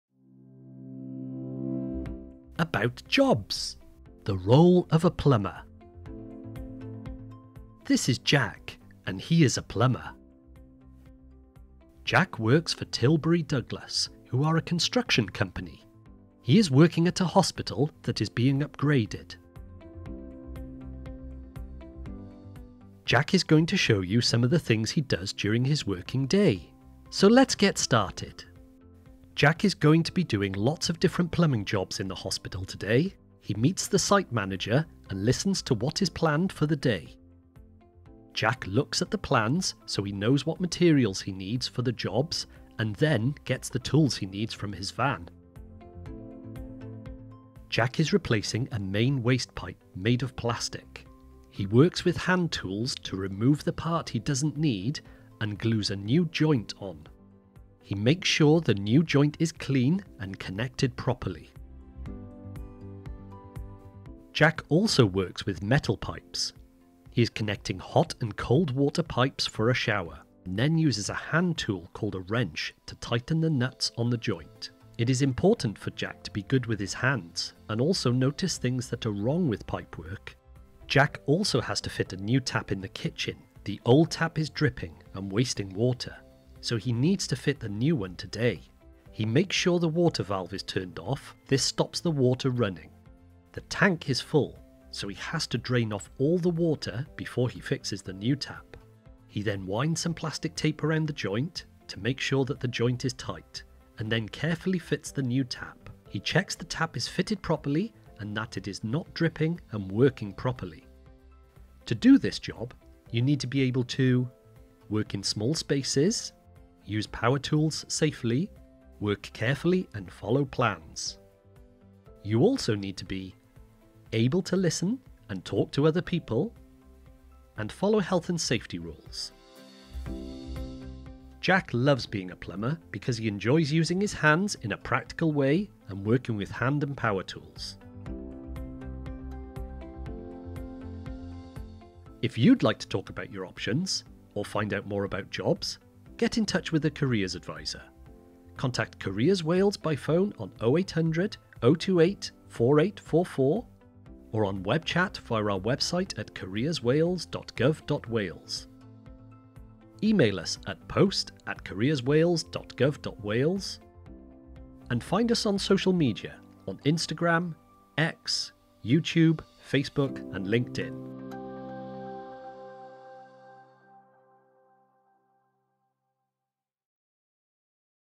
Role of a Plumber
Watch the video to find out some of a things a plumber does.